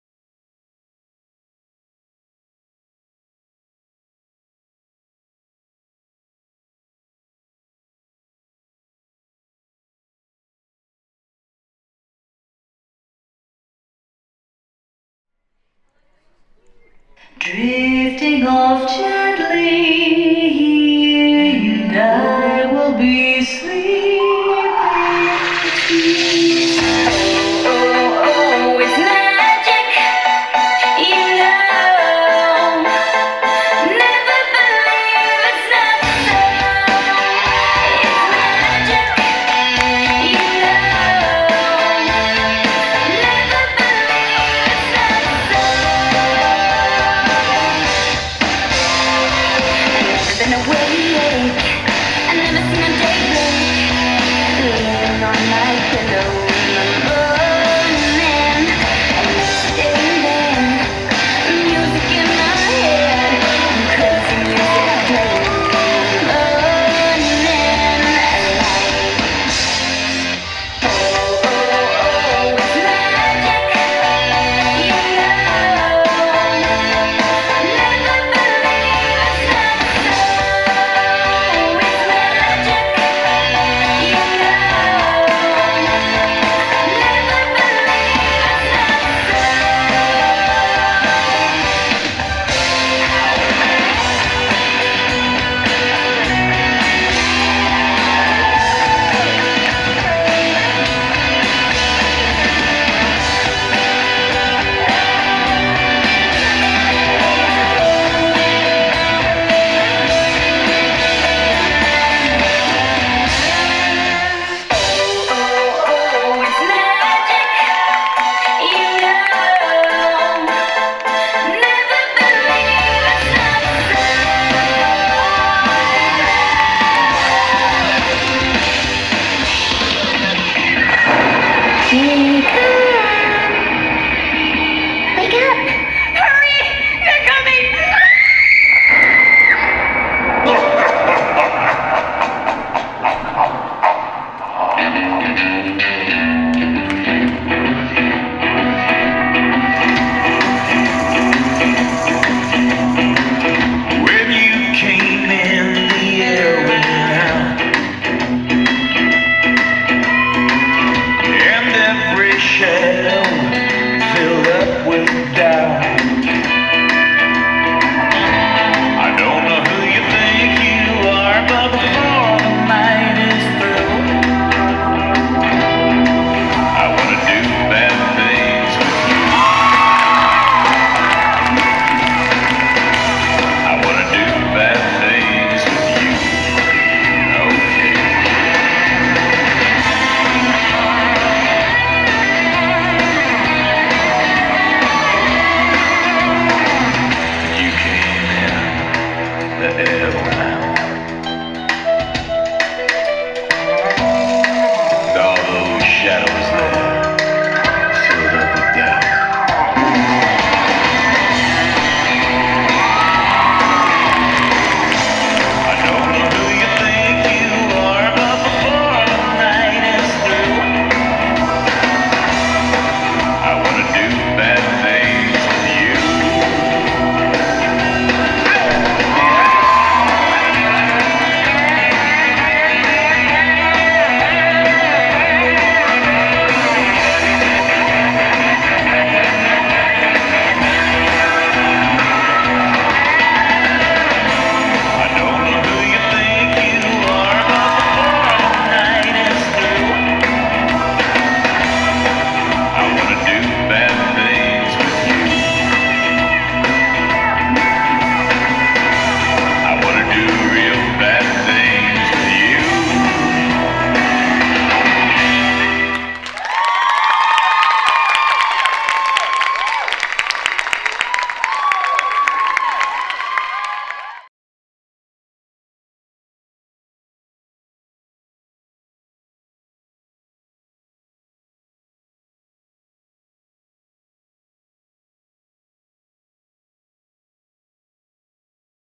PPC 2014 Arloa Reston Entertainment Level 4 Masters Winner
Pole Sport Organization 2014 Pacific Pole Championships - Entertainment Level 4 Masters Champion. March 2, 2014